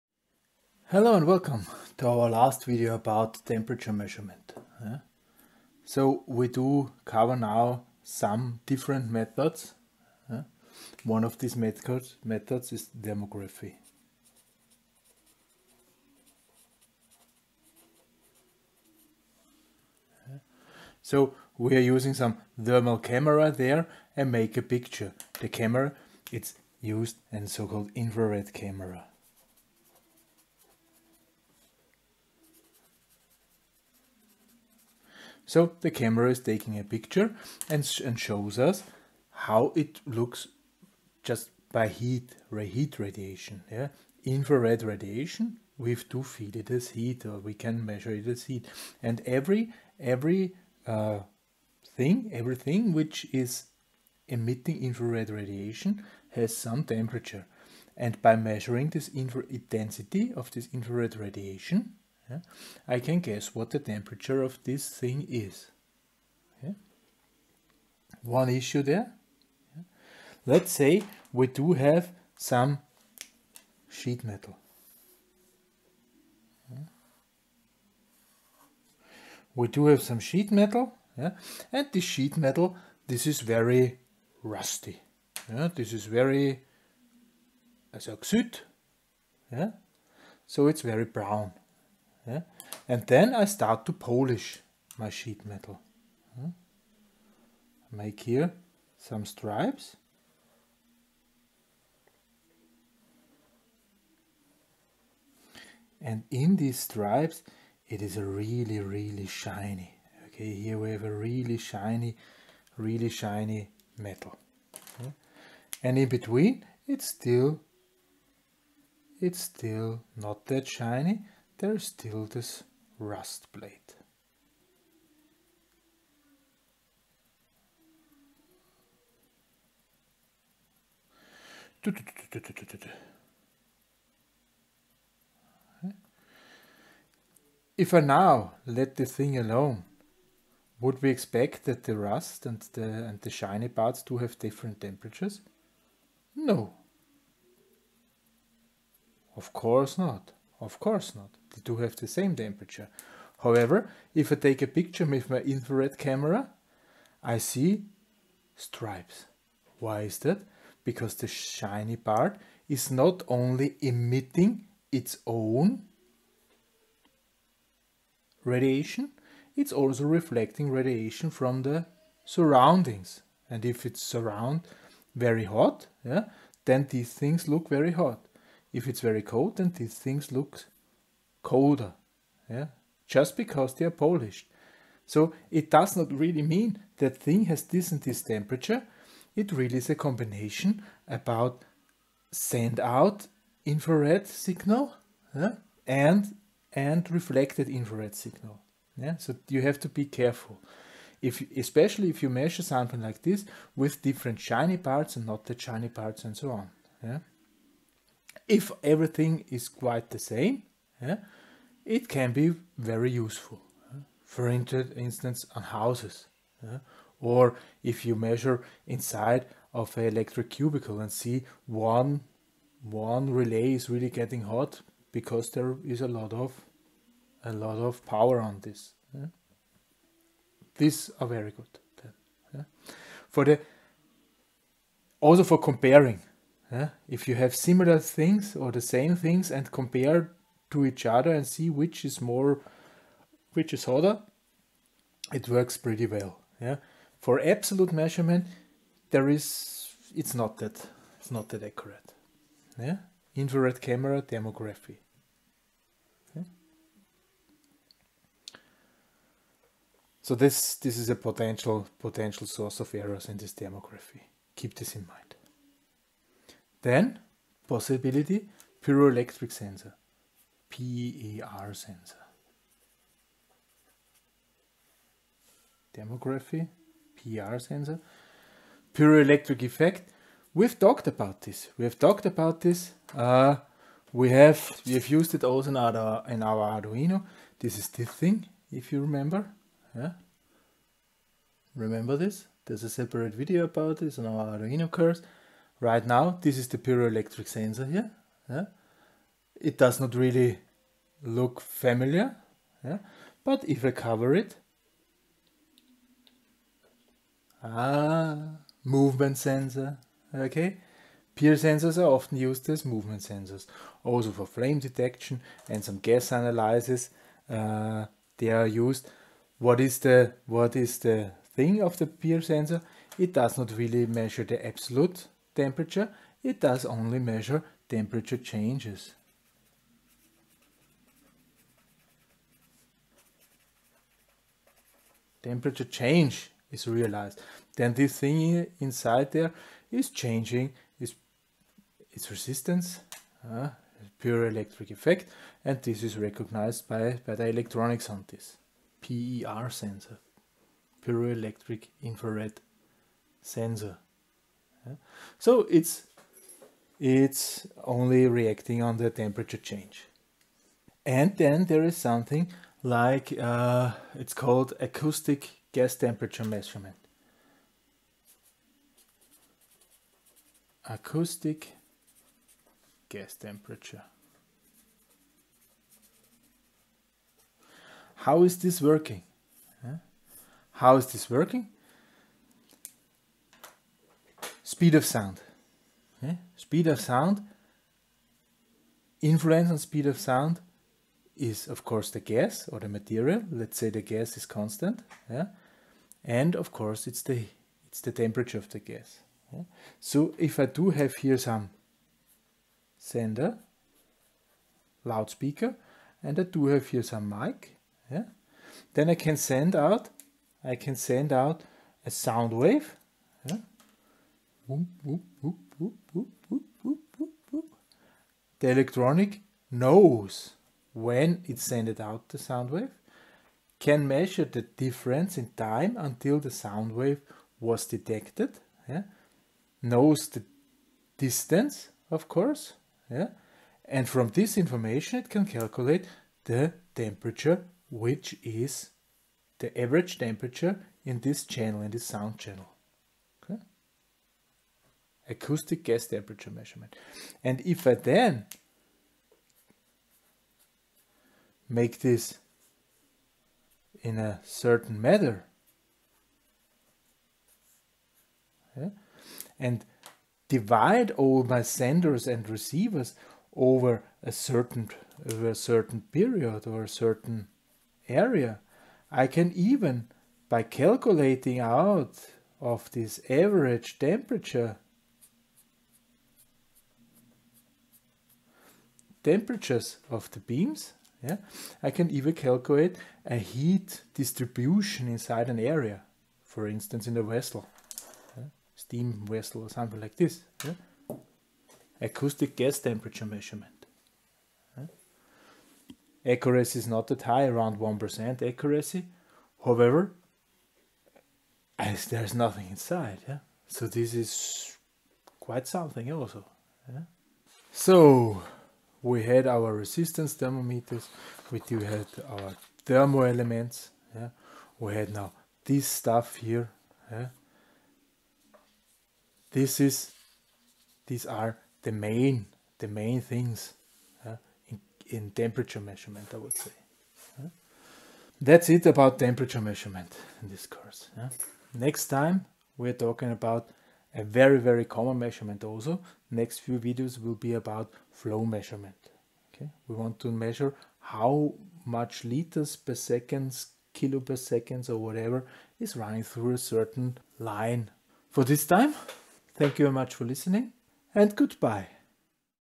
Measure 48: Other Thermal Measurements (Old)
This video deals with different other methodes of thermal measurements. It includes:

thermooptical (infrared-camera)
PIR
acoustic temperature measurement

We will get to know the principle and the strength and weaknesses of these methodes.